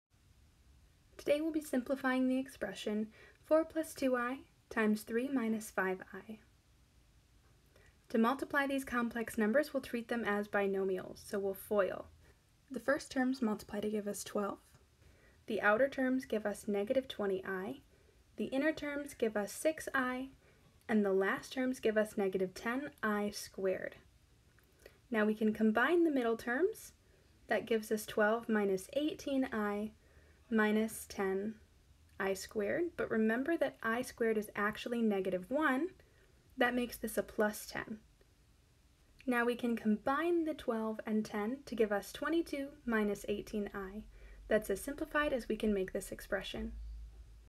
Complex Numbers: Simplify (4 + 2i)(3 - 5i)
Jenna from SVSU Micro Math helps you multiply complex numbers.  Complex numbers are numbers of the form a+bi where i is a square root of negative one, sometimes called an “imaginary number”.

Problem: Simplify (4 + 2i)(3 - 5i)